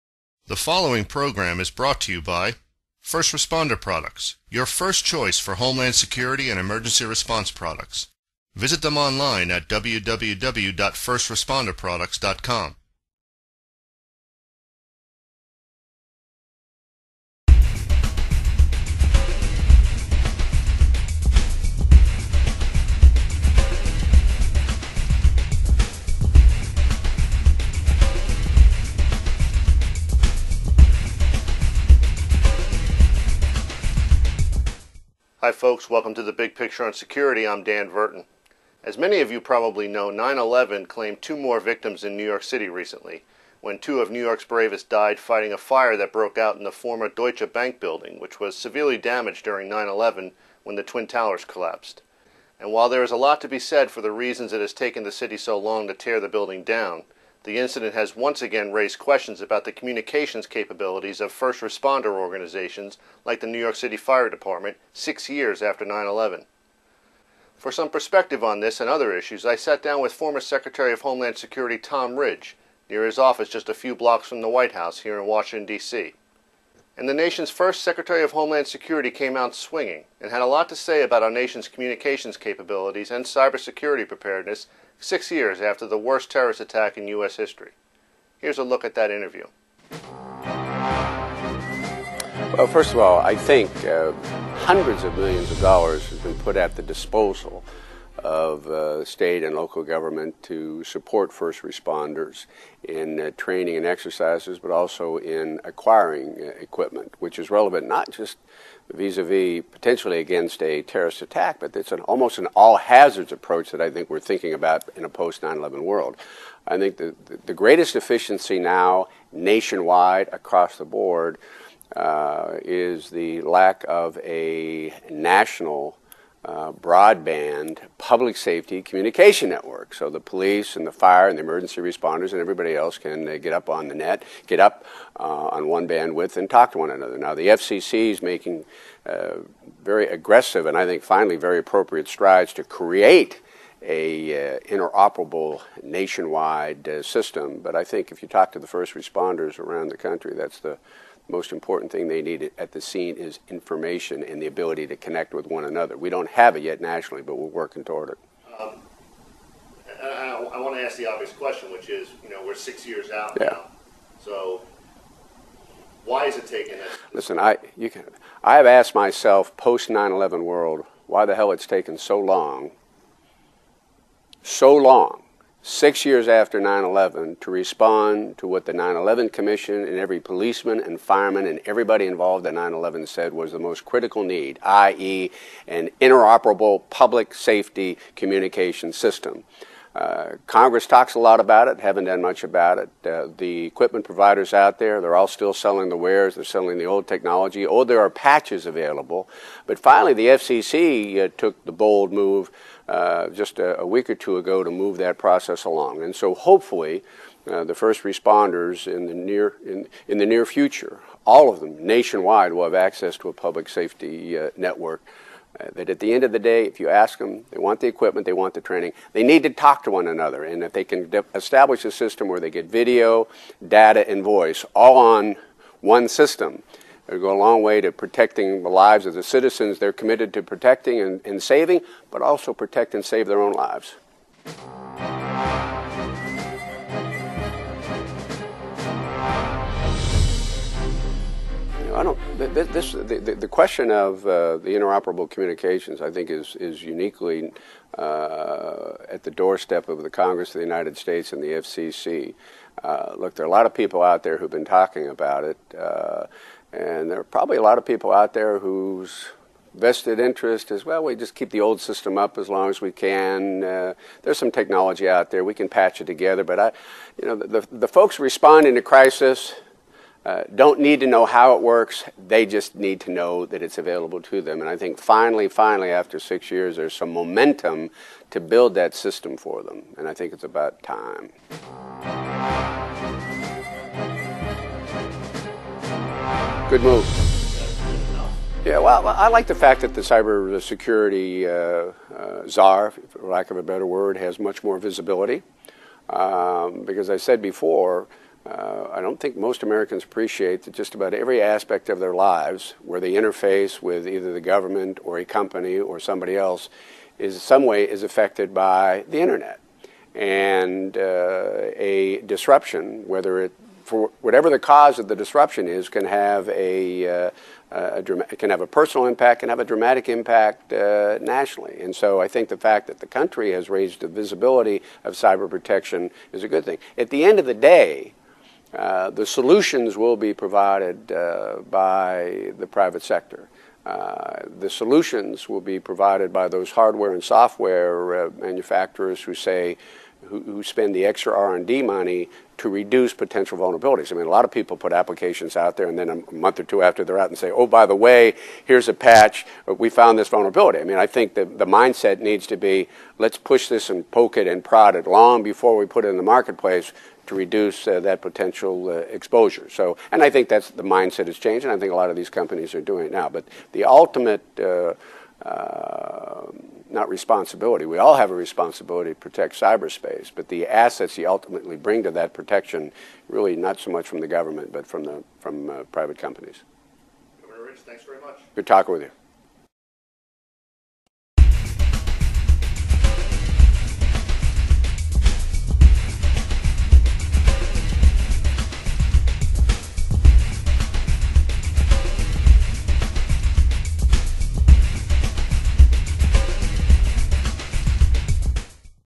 Tom Ridge on Public Safety Communications
In the wake of the deaths of 2 New York City Fire Fighters at an abandoned building near Ground Zero, the issue of interoperable communications for First Responders has once again taken center stage